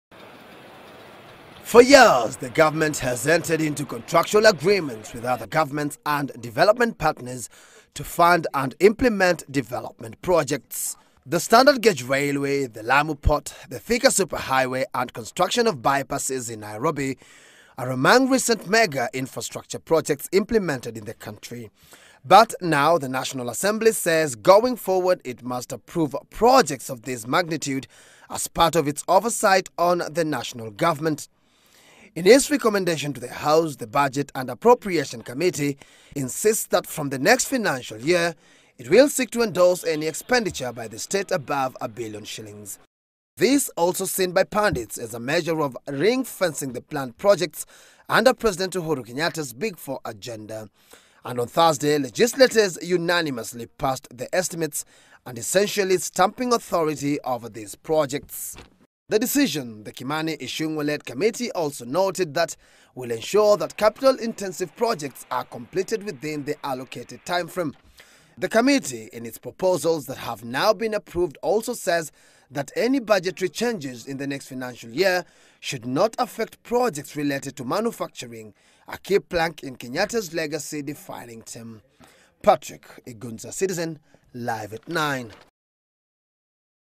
MPs to approve state spending above Ksh 1 Billion
The government will from the next financial year seek approval of the National Assembly of all its projects costing above a billion shillings. This parliament says will help it monitor such capital-intensive projects to ensure they are completed on time.